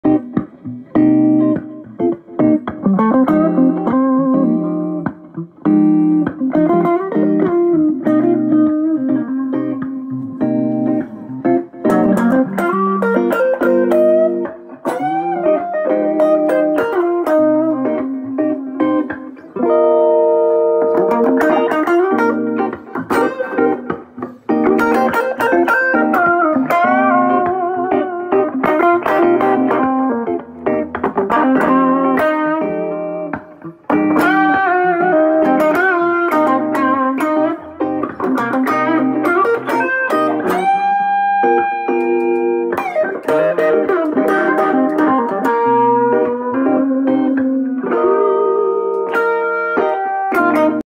Demo of my 1936 Square Amp
I bought this amp from Matt Richards of Square Amps. I love the sound with my Gibson 335